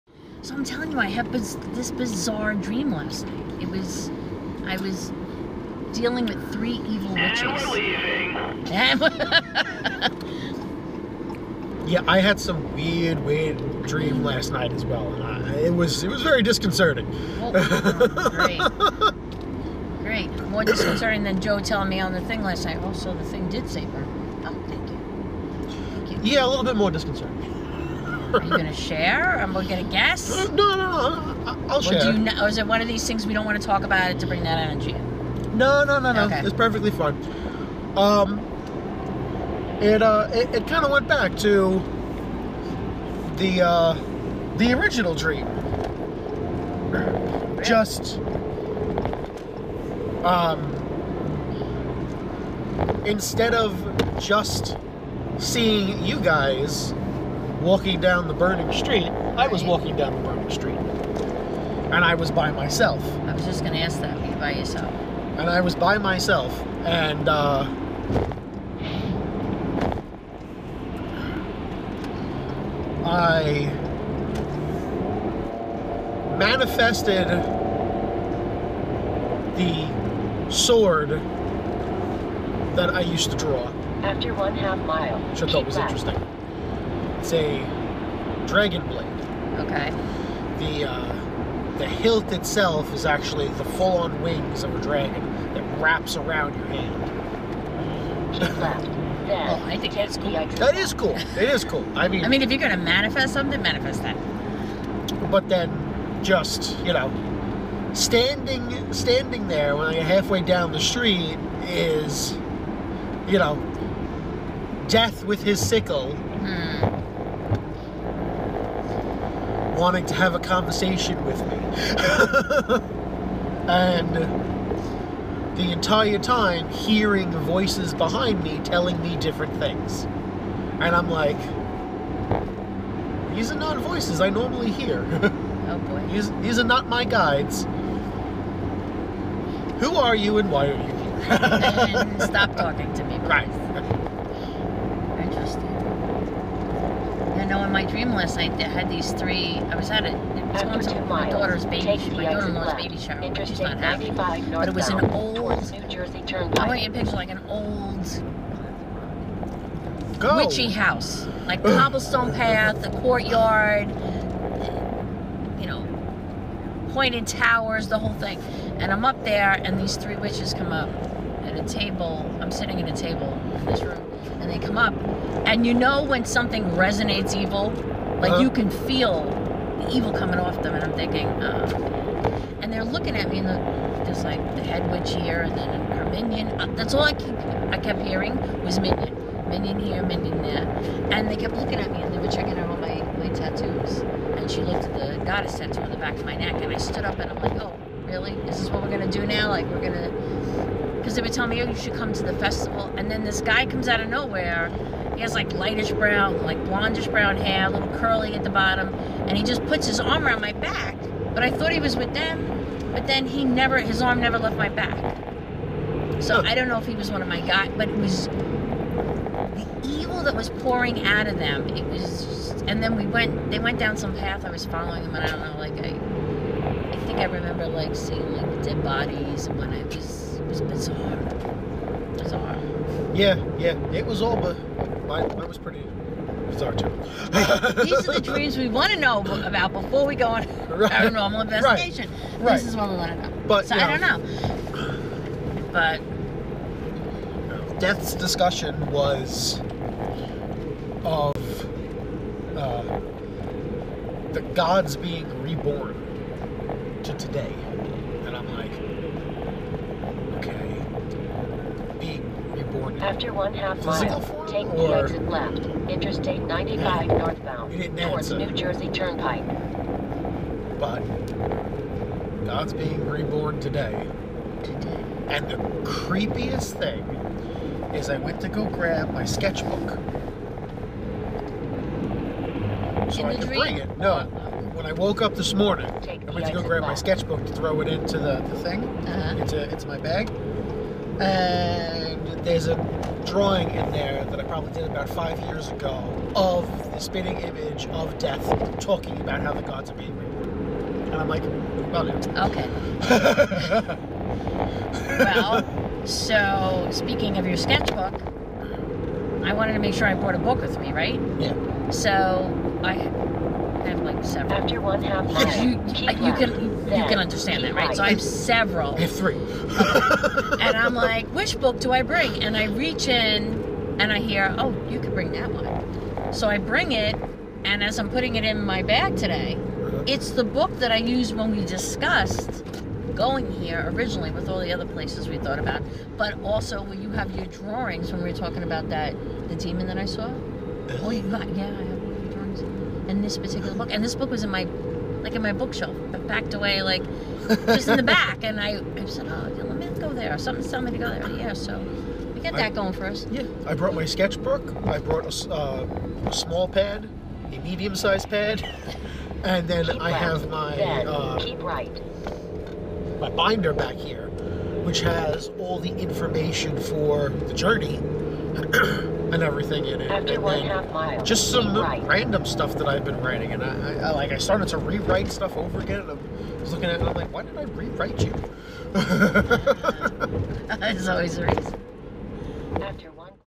Car Shorts: April and Chris's Dreams
On our travels to The Haunted Shanley Hotel, the team discusses various topics in the metaphysical and paranormal field. This segments discussion between April, Chris and Steve about the strange dreams that they had the night before leaving for the investigation.